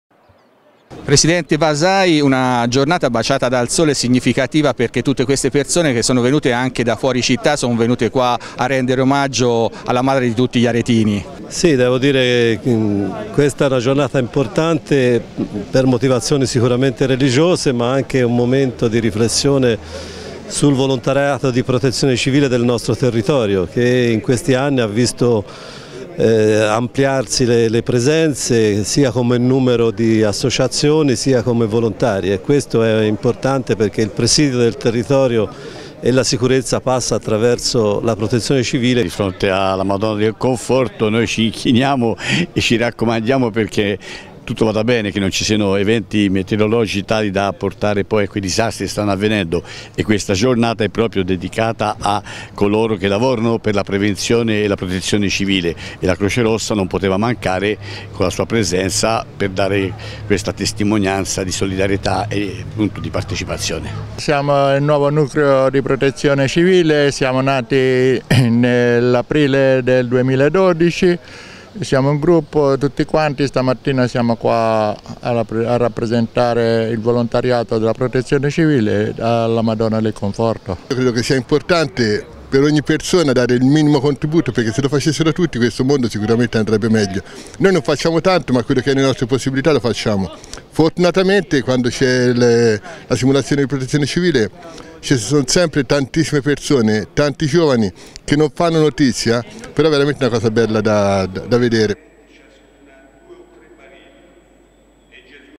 Madonna del Conforto, l'omaggio della Protezione Civile
Prosegue la novena per la Madonna del Conforto. Sabato i volontari della Protezione Civile di Arezzo hanno reso omaggio alla Patrona.
Le interviste a Roberto Vasai, presidente della Provincia di Arezzo, Carlo Cigna, presidente della Croce Rossa Arezzo, e ad Antonello Felici e Alano Maffucci, volontari del nucleo Associazione Nazionale Carabinieri.
Servizio di Enrico Donati. Riprese di Gianni Stanganini.